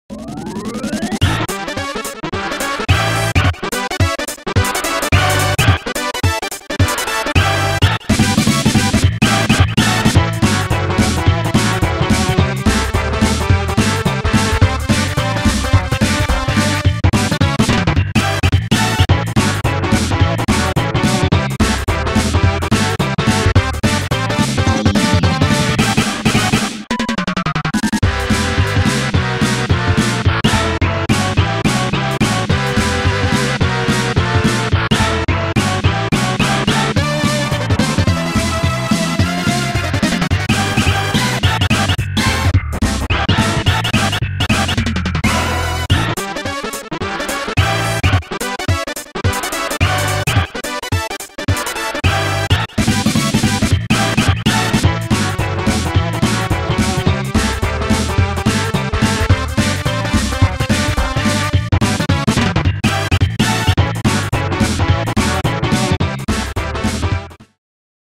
Dingo Draws: Dingo X TMNT Crossover 🐢🐢🍕🐕🐢🐢 [Time Lapse Drawing]
A fun little speed art video for my Tmnt and Dingo Drawing! 🐢🐢🍕🐕🐢🐢